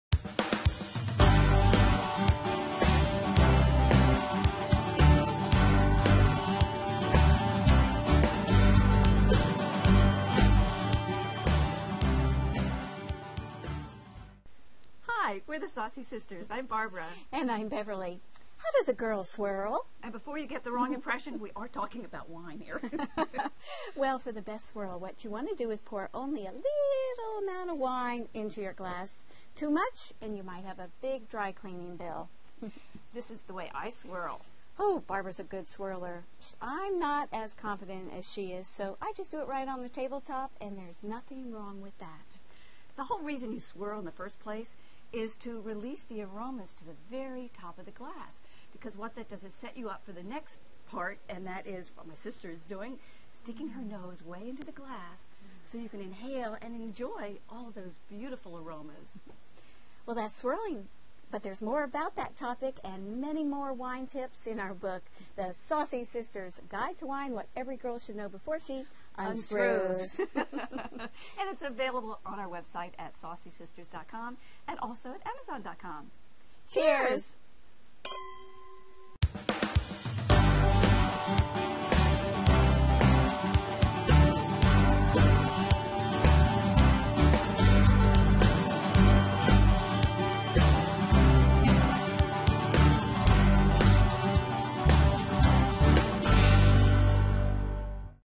The Saucy Sisters talk about Swirling Wine
Beverly Pittman and Barbara Nowak, The Saucy Sisters, aka wine experts with a "twist" (of humor, that is) explain how to swirl wine and why we do it.

Their latest book, The Saucy Sisters Guide to Wine, What every girl should know before she UNSCREWS!